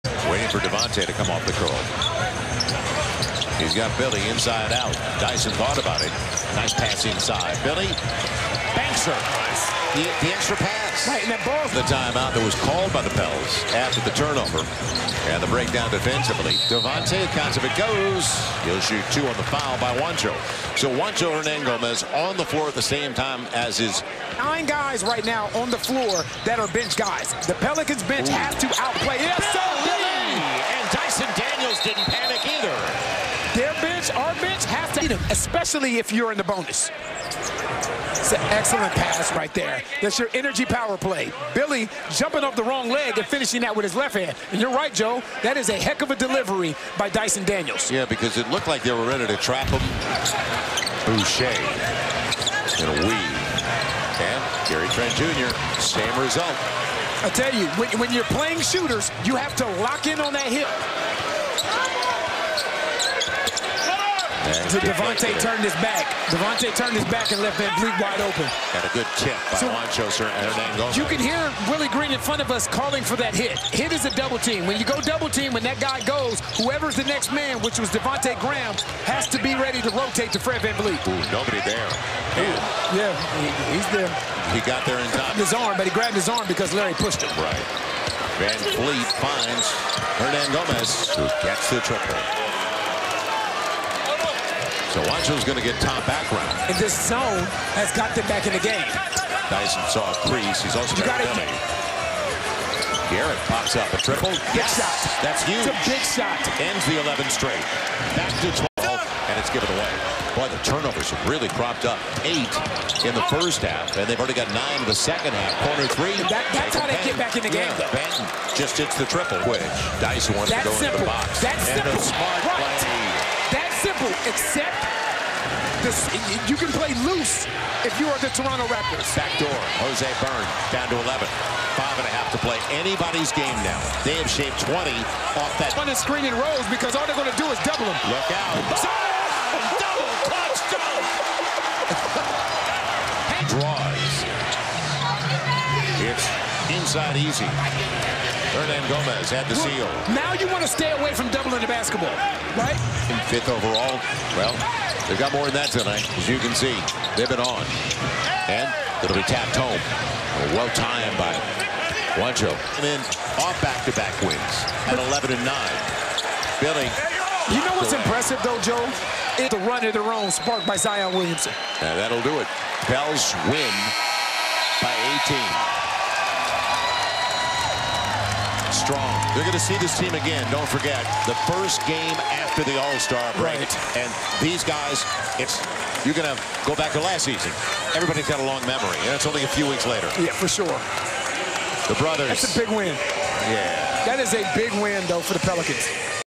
Willy Hernangómez vs Juancho Hernangómez (30 - 11 - 2022)
New Orleans Pelicans vs Toronto Raptors (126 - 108)
Willy Hernangómez: 11 min, 4 pts, 3 reb, 2/4 FG
Juancho Hernangómez: 23 min, 9 pts, 3 reb, 3 ast, 4/6 FG, 1/3 3PT